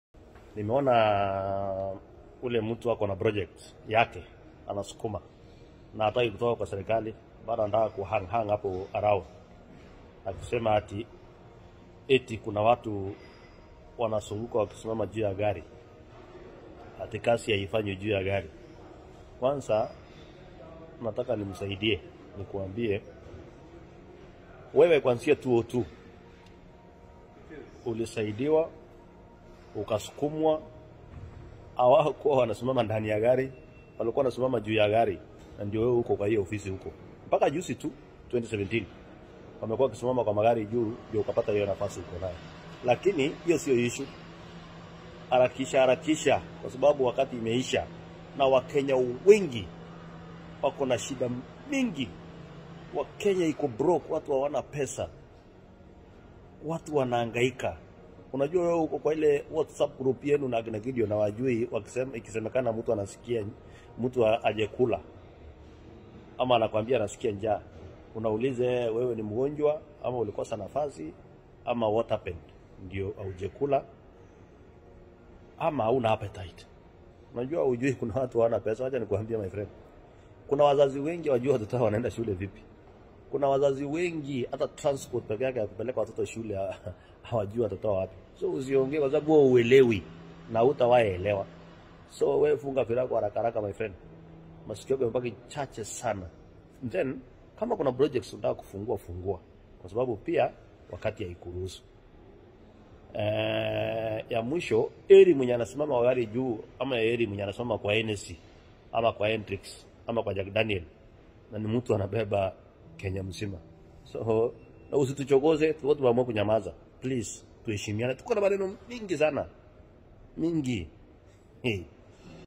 OSCAR SUDI: Responds to PRES. UHURU KENYATTA 🔥🔥🔥 | Usituchokoze na Tulikua Tumenyamaza
Ujumbe wangu kwa rais Uhuru Kenyatta. Heri mwenye anasimama Juu ya gari ama mwenye anasimama Juu ya Hennessy na Jack Daniels?
Usituchokoze na tulikua tumenyamaza!